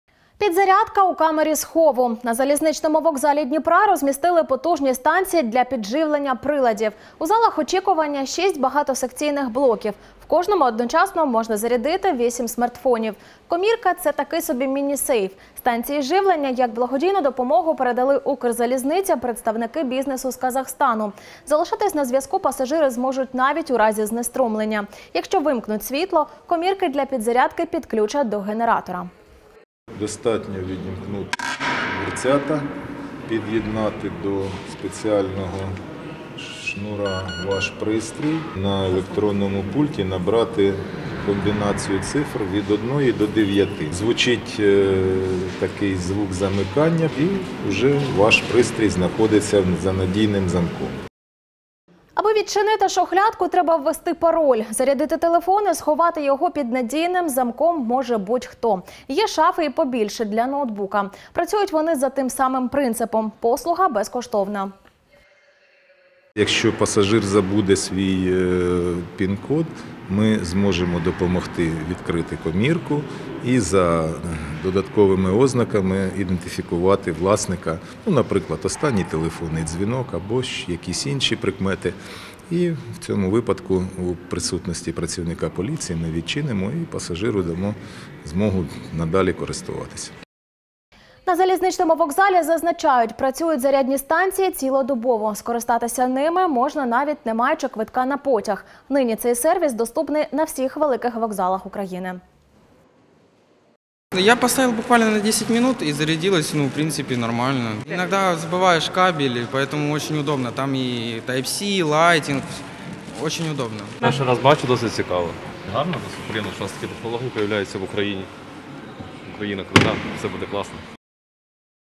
У Дніпрі на залізничному вокзалі встановили безкоштовні «зарядні» станції для ґаджетів
У Дніпрі на залізничному вокзалі встановили «зарядні» станції для гаджетів. Послуга безкоштовна, підзарядити телефони та ноутбуки можна цілодобово.